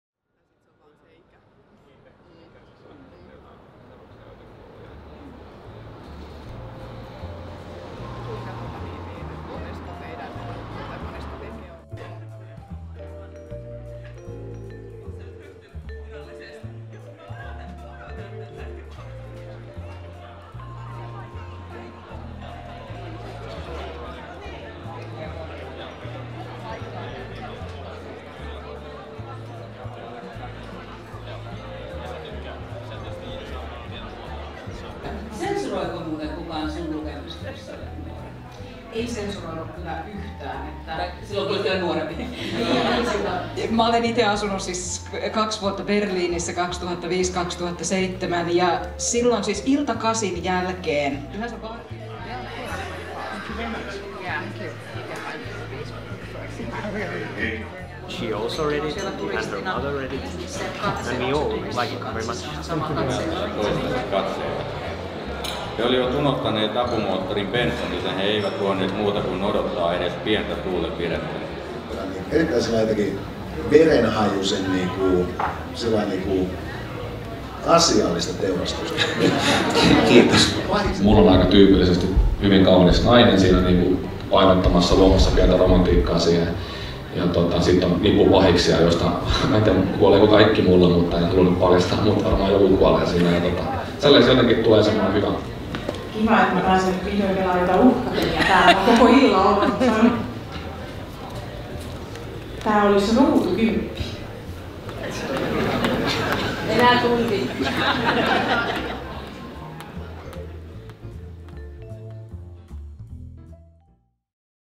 Dekkarifestivaalit 17.9.2016 @ Casino Helsinki
Toista kertaa järjestettävä dekkarifestivaali veti Helsingin Casinon salin täyteen paitsi vieraita, myös eturivin kirjailijoita Suomesta ja maailmalta.

Paikalla mm. Leena Lehtolainen, Markus Ahonen, Paula Arvas, Tapani Bagge, Joël Dicker, Marko Hautala, Risto Isomäki, Saara Kesävuori, JP Koskinen, Marko Lönnqvist, Matti Remes, Markku Ropponen, Christian Rönnbacka, Max Seeck, Salla Simukka, Jarkko Sipilä ja Vera Vala.

Tuotanto: Jaakko Tiikasalo / Panoptik